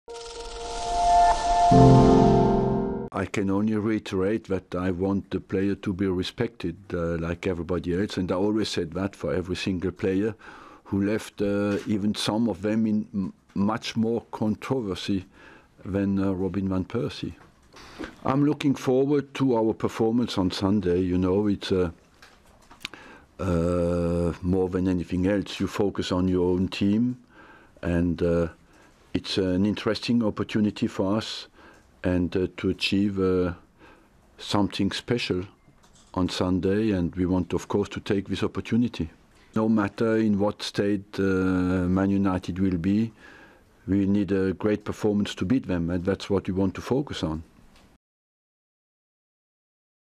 Arsenal fans should respect Robin van Persie, says Arsène Wenger
Click here for the latest British & Irish Lions Brick by Brick

Arsène Wenger tells reporters that he wants former Arsenal striker Robin van Persie to be respected when the Dutchman returns to the Emirates for the first time with Premier League champions Manchester United. Wenger looks forward to Sunday's important Premier League encounter where Arsenal will give the champions a guard of honour.